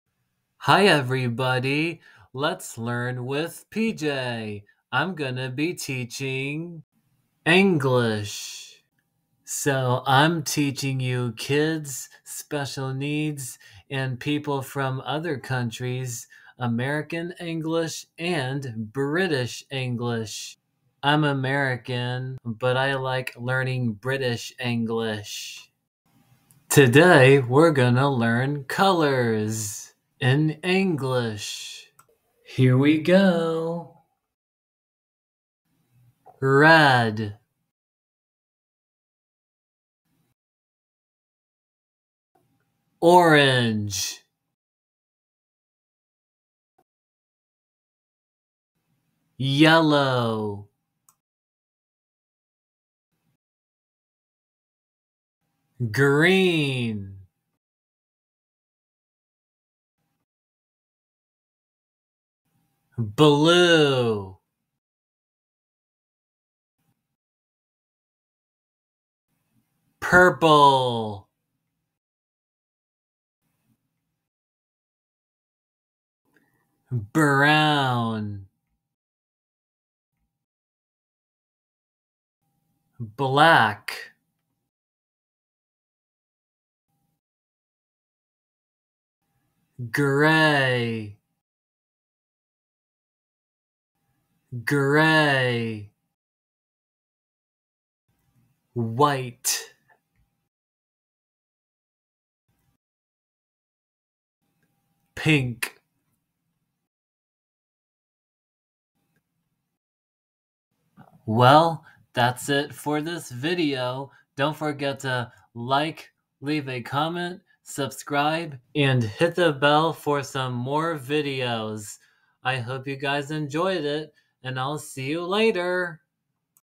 Why Colours Are Different In English? (How to Pronounce Colours In English)
Do you want to learn to pronounce colours in English? That's great! This video is perfect for you, you've come to the right place. Let's learn how to pronounce colours in English.

List of Colors

What are the colours?

 - Red
 - Orange
 - Yellow
 - Green
 - Blue
 - Purple
 - Brown
 - Black
 - Gray (American English)
 - Grey (British English)
 - White
 - Pink

List of Colours in English

Including British and American English. (4 Example: In American English, color spells COLOR. And In British English, colour spells COLOUR.)